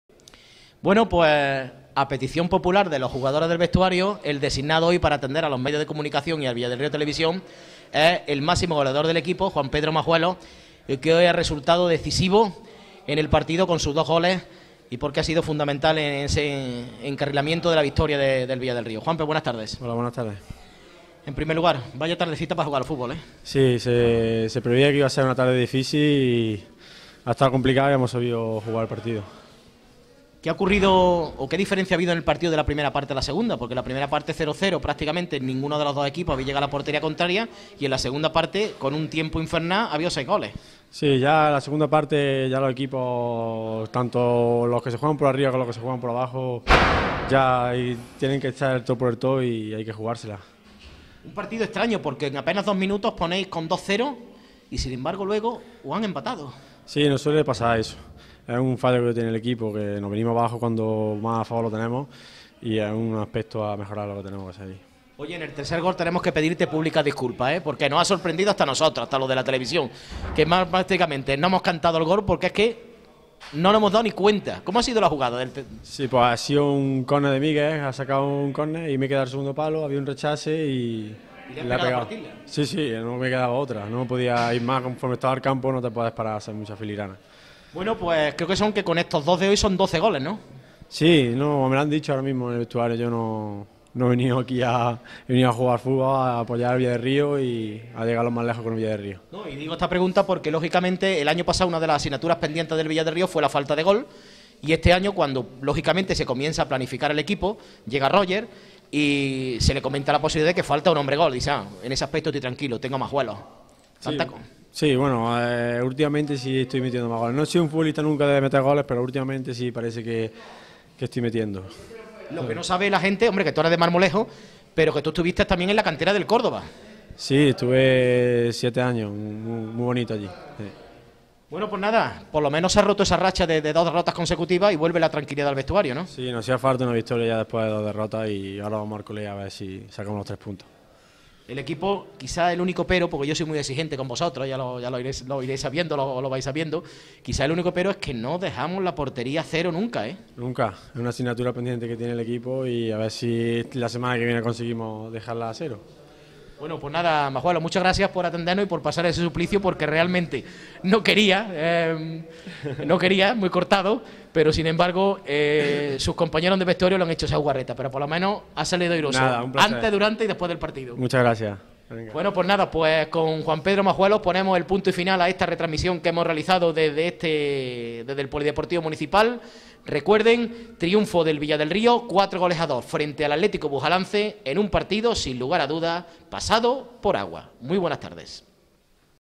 Entrevista a Juan Pedro Majuelos 12 goles ya en su haber. FUTBOL villadelrio-bujalance9feb2014--
entrevista de Francisco Jose Garcia para la TV Local.